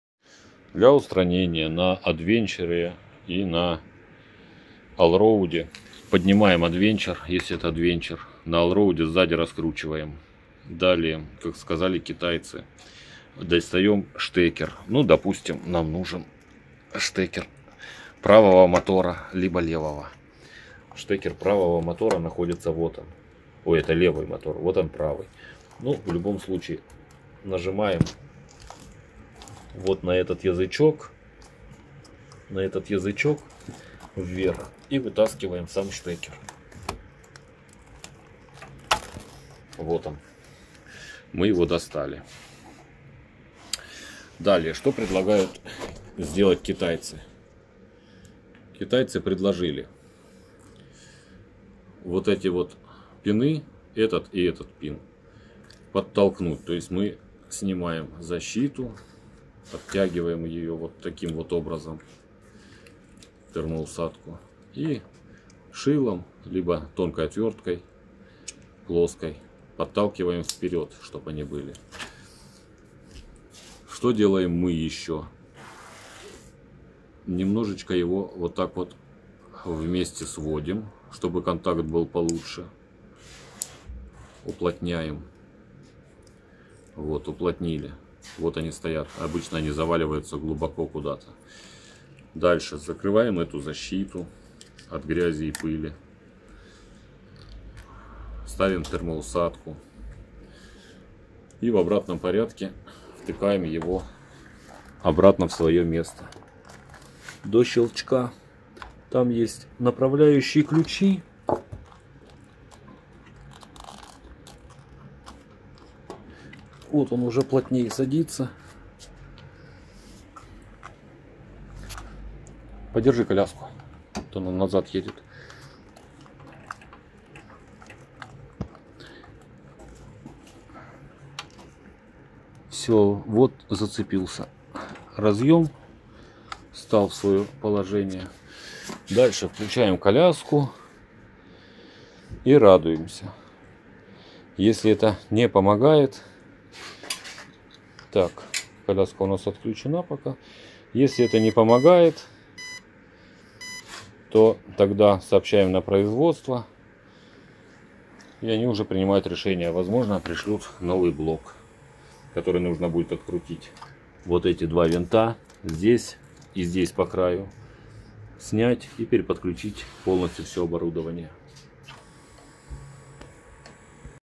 РЕШЕНИЕ ПРОБЛЕМЫ ОТ ПРОИЗВОДИТЕЛЯ
При ошибках L8, R8. C5, C2 необходимо поправить пины левого и правого моторов , поджать их .